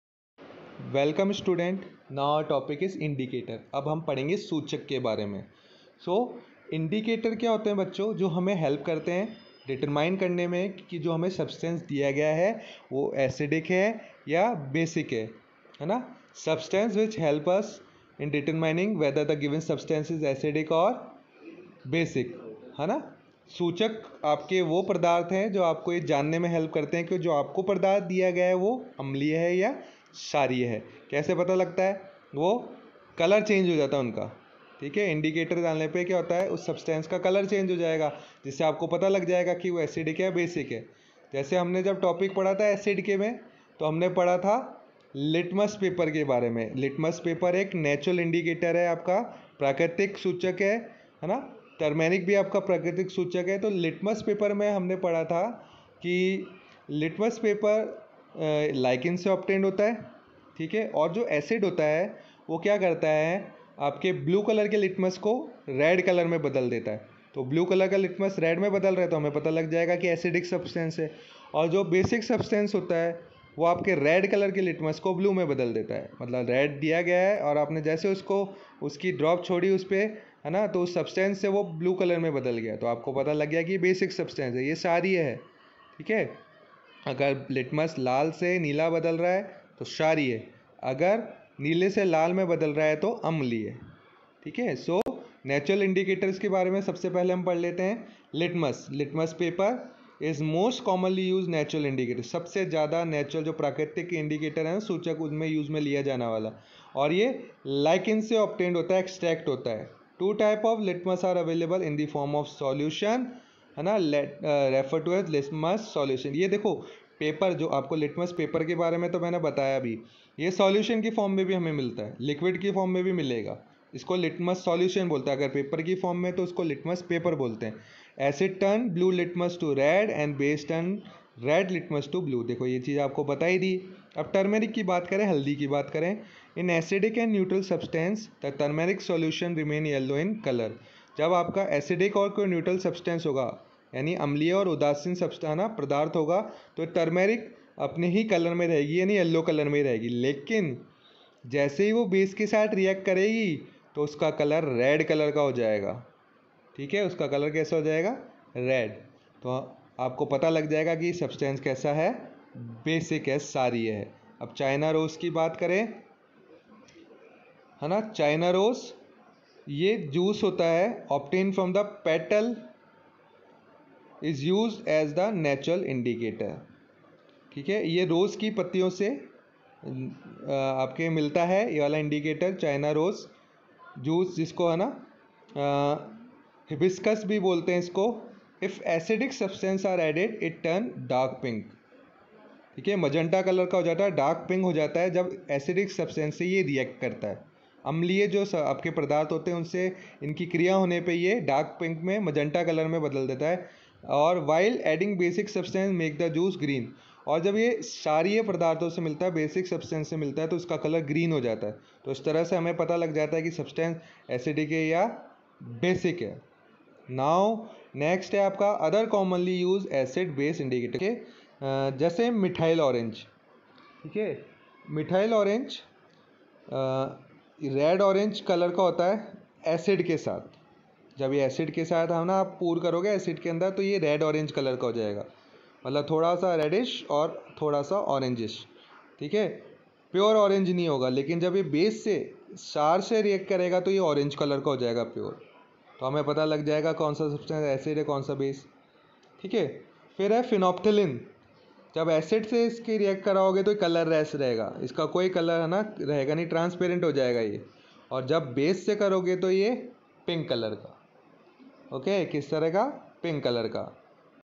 Class 7 science ncert | Chapter 5 | Acids and Bases | Topic Indicators
In this video Indicators is explained for both Hindi and English medium students

link to change resolution of video on YouTube

Resolution